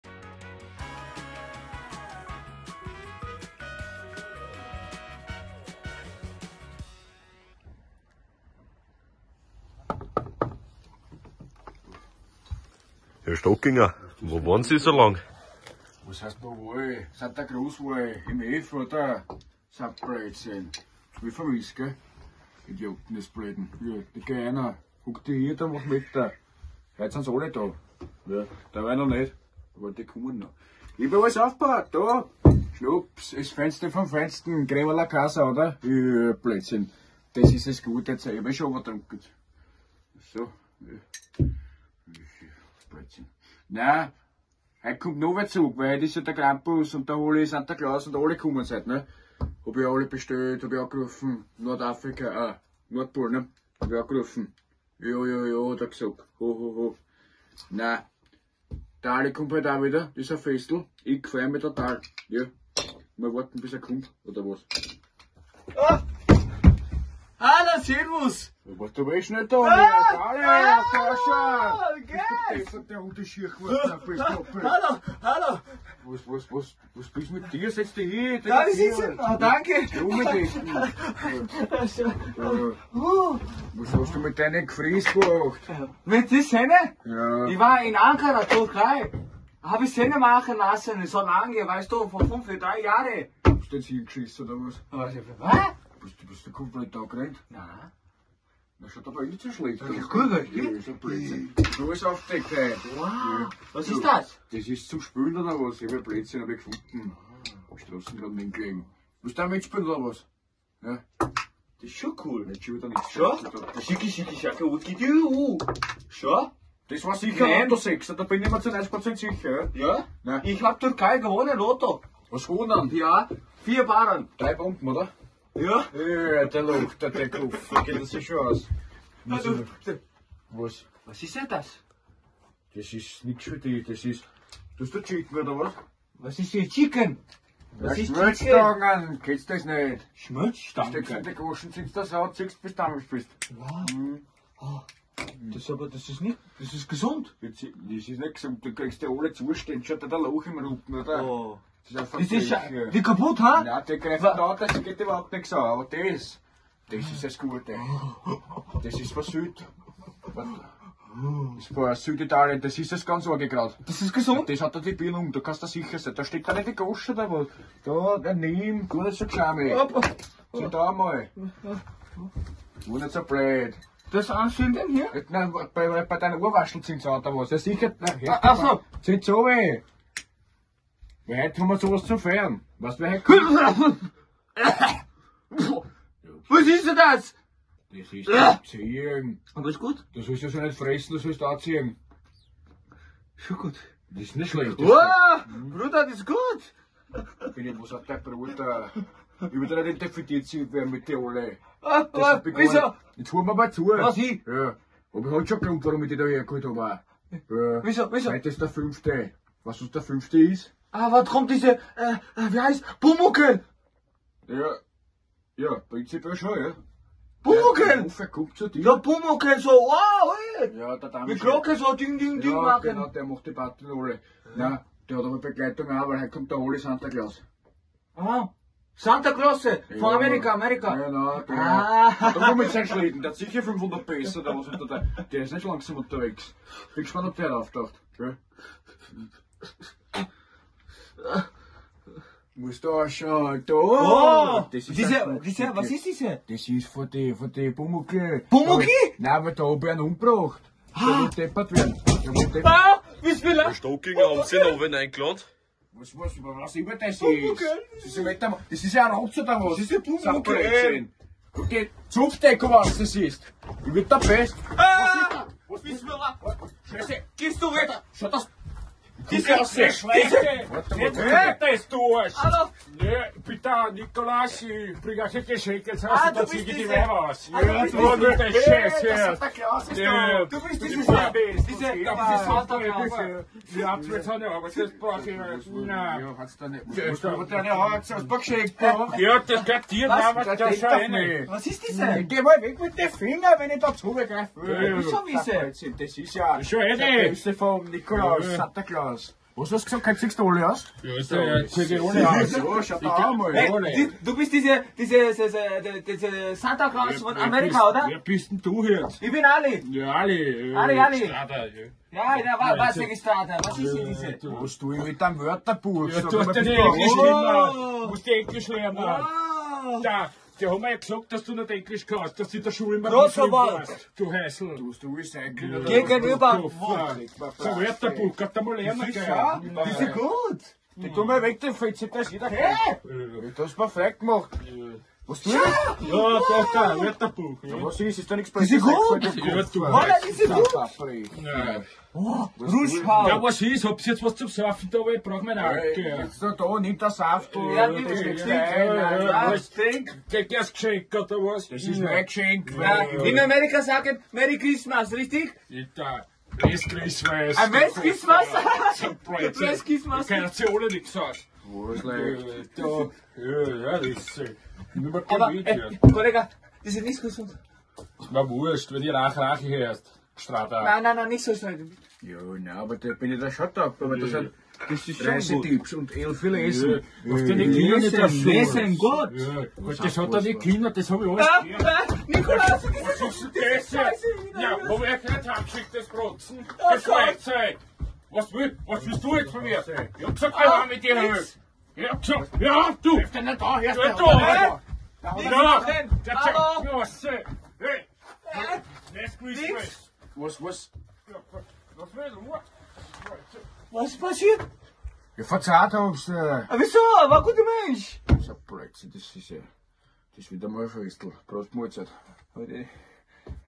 Peter Stockinger | Besuch vom Krampus | Folge 7 (Combeback Special)
In dieser Folge, können wir Peter Stockinger wieder beim Tratschen mit Herrn Ali beobachten. Doch am Krampustag gibt es immer Überraschungen